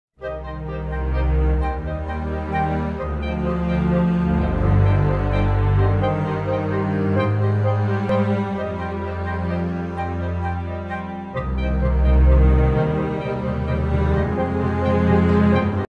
LEGO Technic Land Rover Defender (42110) 🚘 🏔
A look at the LEGO Technic Land Rover Defender (in stop motion).